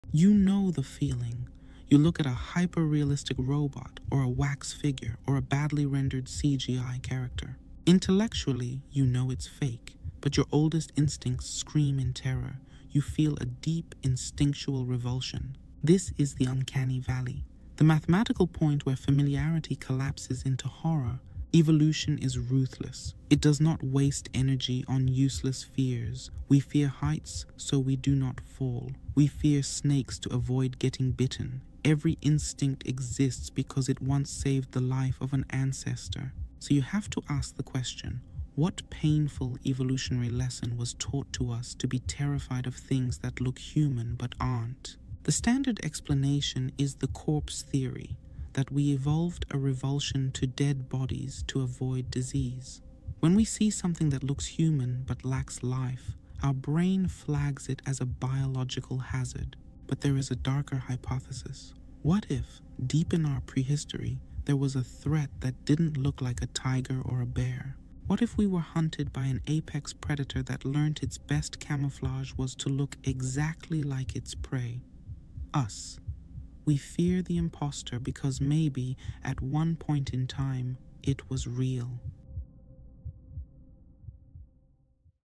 The Uncanny Valley
Evolution is ruthless. It does not waste energy on unnecessary fears. We fear heights to avoid falling. We fear snakes to avoid getting bitten, etc.

So why do we fear things that look human, but aren't?

The "Uncanny Valley" suggests a glitch in our facial recognition. But the "Malignant Mimic" theory suggests something darker:

If we are hardwired to spot the impostor, it means that at some point in human history, the impostor was real. We were hunted by something that wore our face.